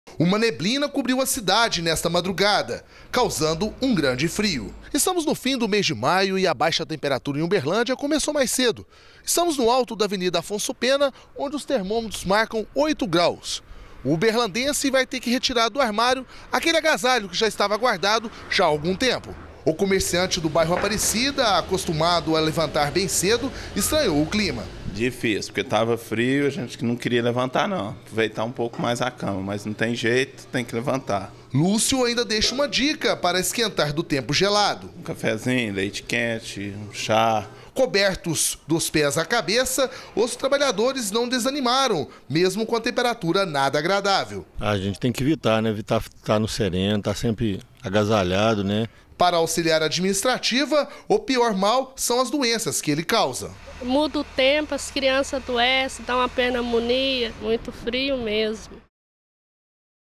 Uberlândia registra madrugada mais fria do ano, com 8ºC
Jornal da Vitoriosa e Portal Uipi de 28/05/14